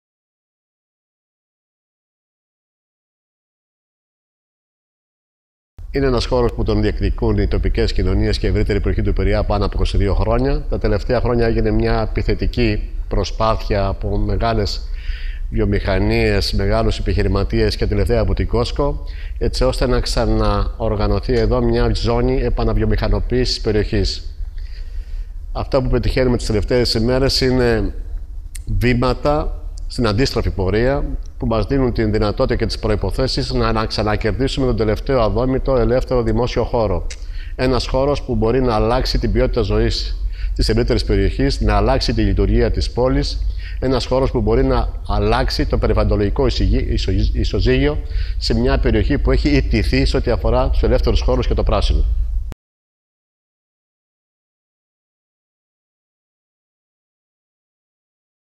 Περιβαλλοντική παρέμβαση στη Δραπετσώνα -Γιώργος Γαβρίλης
Λιπάσματα Δραπετσώνας: Περιβαλλοντική παρέμβαση με βάση τις ανάγκες των κατοίκων
Σήμερα Σάββατο 31/10 κάτοικοι της Δραπετσώνας μαζί με τον δήμαρχο Κερατσινίου-Δραπετσώνας, Χρήστο Βρεττάκο, πραγματοποίησαν επίσκεψη-δενδροφύτευση στο χώρο των πρώην Λιπασμάτων διεκδικώντας την έκταση για τους κατοίκους.
Όπως μας είπε ο δήμαρχος χάρις στη δέσμευση του πρωθυπουργού Αλέξη Τσίπρα και τις ενέργειες της κυβέρνησης ο χώρος οδεύει προς το να αποδοθεί στους κατοίκους της περιοχής και όχι σε ιδιώτη «επενδυτή».
Στο φακό του left μίλησαν ο Χρήστος Βρεττάκος, δήμαρχος Κερατσινίου-Δραπετσώνας και ο Γιώργος Γαβρίλης, Αντιπεριφερειάρχης Πειραιά.